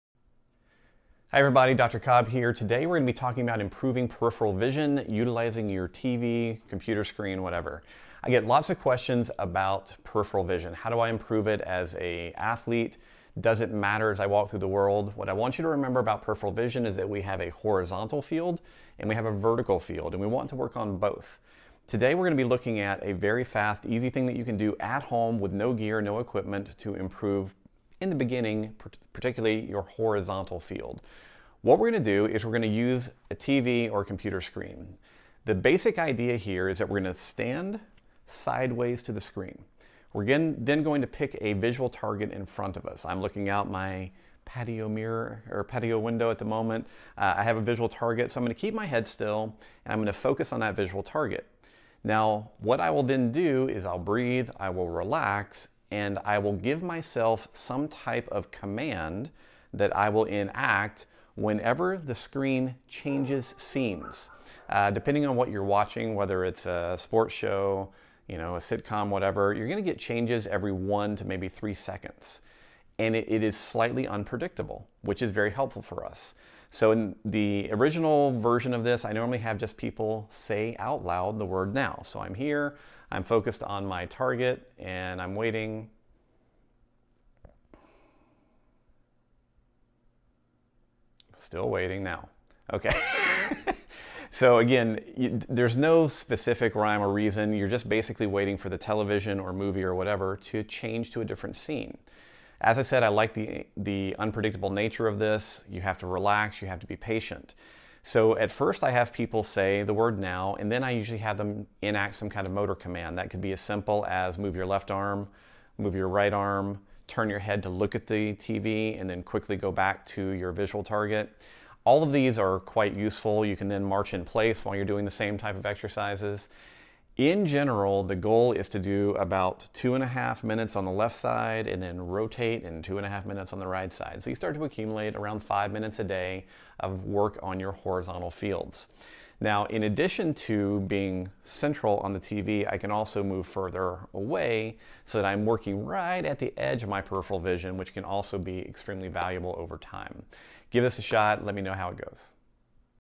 Improve Your Peripheral Vision At Home (No Equipment Necessary!)
In this video, we are going to show you an easy way to train your peripheral vision for better attention, awareness, and relaxation. All you need is your TV, so put on your favorite sports channel, sitcom, or movie, and give this a try!

Video Highlights:
- Introduction to peripheral vision
- Using your TV to train peripheral vision
- Adding verbal and movement responses to enhance the drill